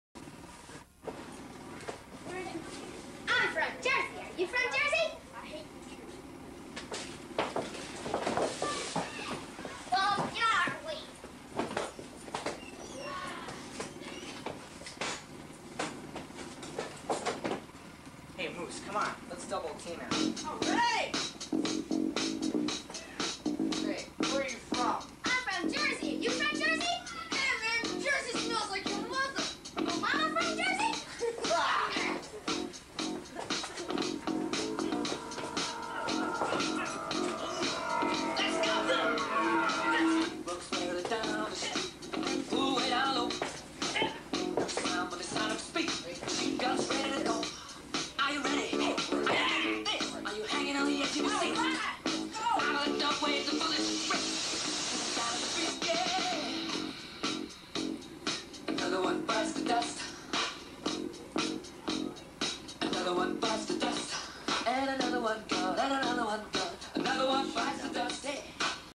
1980 Patrol Skit | "I'm from Jersey. You from Jersey?" | BSA Troop 61 | Milwaukee, WI
Scouts in this video include:
John Cerbins
Kurt Gardener
Brian Moscicki
John Wood